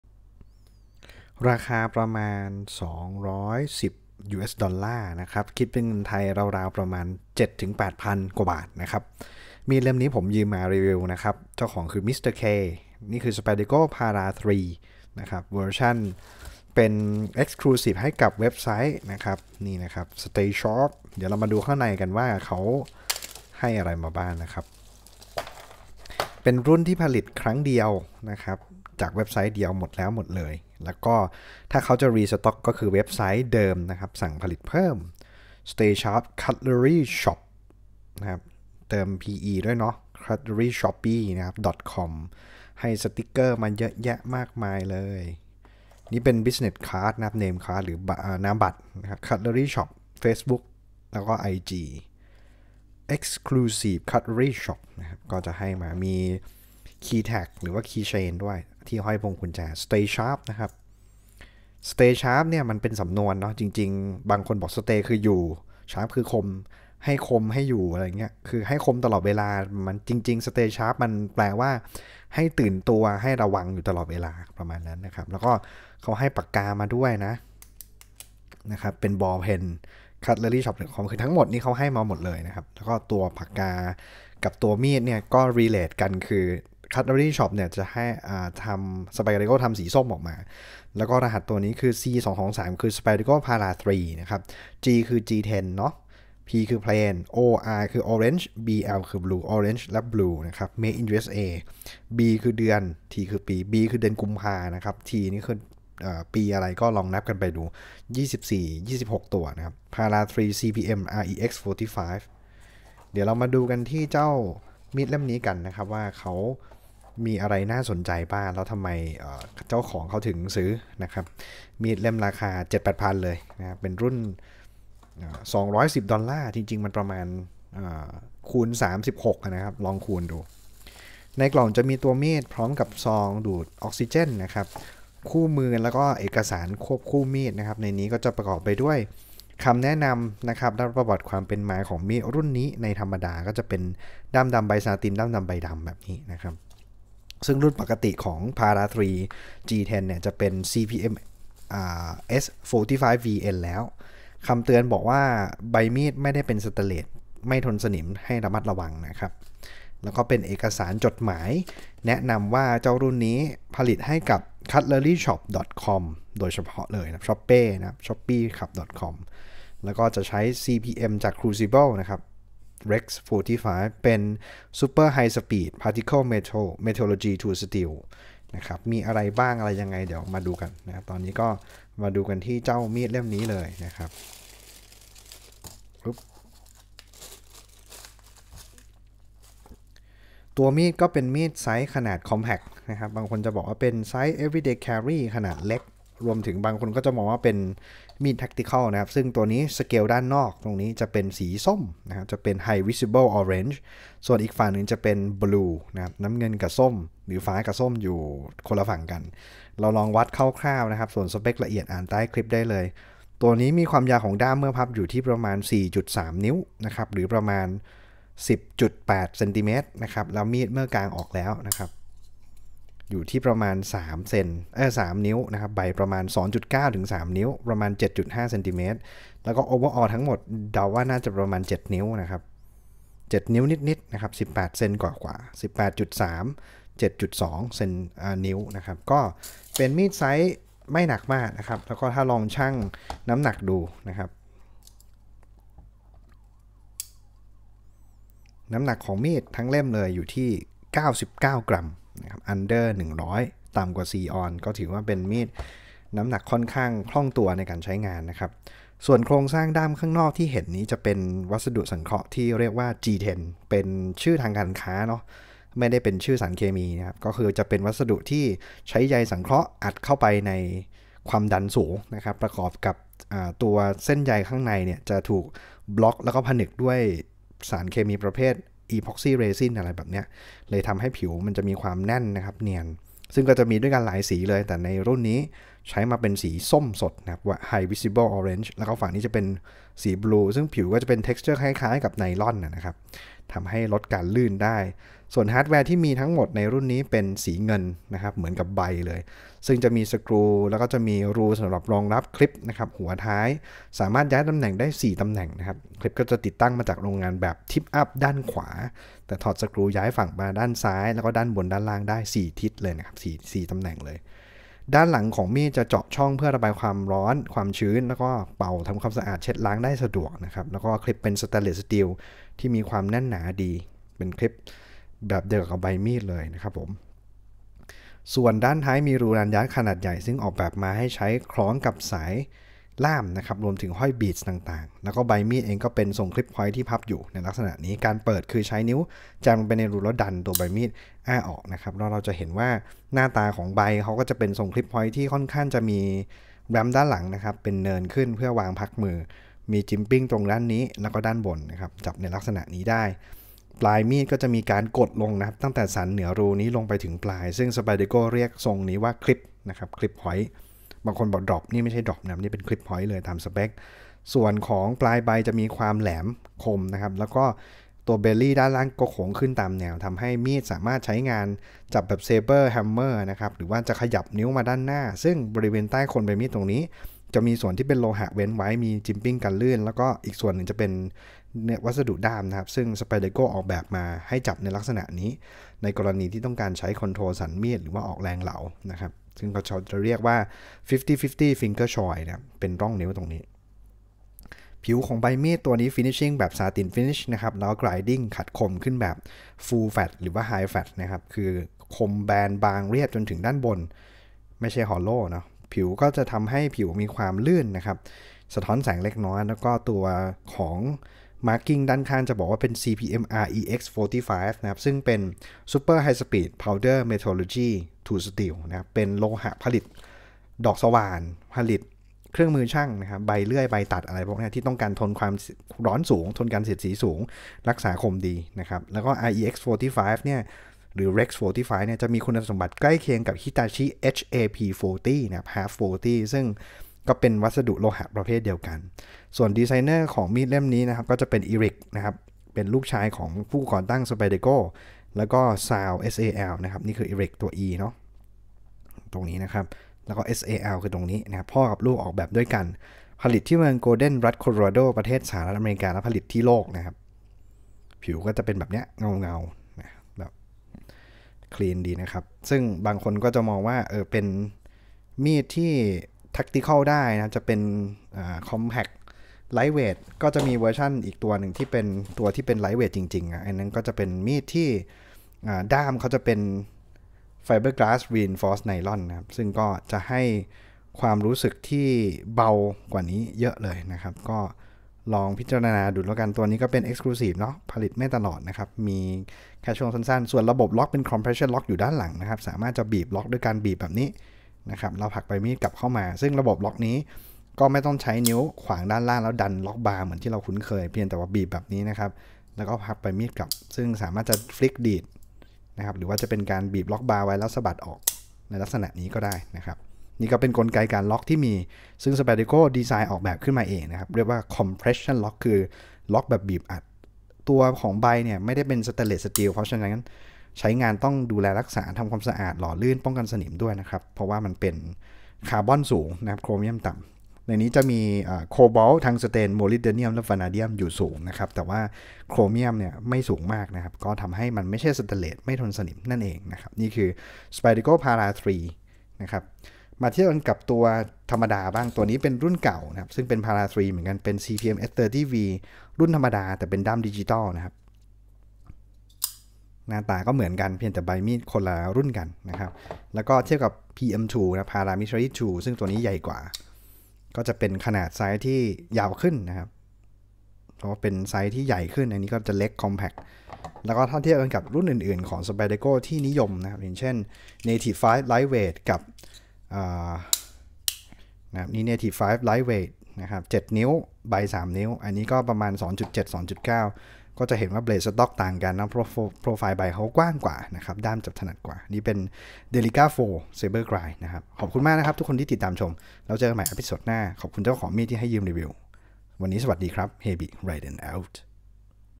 SPYDERCO PARA 3 CPM REX45 SATIN ORANGE BLUE ยืม ผลิตให้ Cutlery Shoppe Exclusive
$210.00 MSRP Para® 3 Orange / Blue G-10 CPM REX 45 Exclusive C223GORBL
This unique knife was produced for and available exclusively through Cutlery Shoppe ) and their sales channels.

The first defining feature of this knife is its blade. Instead of the standard blade steel used on this model, it is crafted from Crucible® CPM® REX® 45—a super-high-speed particle metallurgy tool steel enriched with large volumes of cobalt, tungsten, molybdenum, and vanadium. Its high cobalt content increases the steel's attainable hardness, while its vanadium carbides provide high wear resistance, fine grain size, and increased toughness. The satin-finished PlainEdge™ blade of this knife is full-flat ground for superior edge geometry and proudly includes a fully accessible Trademark Round Hole™ for swift, ambidextrous, one-handed opening.

Equally unique, this elite knife features a two-toned G-10 handle. The obverse (front) scale is a high-visibility orange, while the reverse (back) scale is blue. Both scales have a non-slip peel-ply texture and are precision machined to accept the nested stainless steel liners that provide structural strength and serve as the heart of the knife's high-strength Compression-Lock® mechanism. A polished four-position hourglass clip completes the package, offering a choice of tip-up or tip-down, left or right-side carry.

Proudly offering a one-of-a-kind combination of features, this Cutlery Shoppe Exclusive version of the Spyderco Para 3 is certain to become a highly prized collector's item.

Steel CPM® REX® 45
Closed Length 4.29" (109mm)
Edge Length 2.57" (65mm)
Handle G-10
Clip Position Ambi
Tip Carry Position 4-position
Lock Type Compression
Grind Full-Flat
Sheath N/A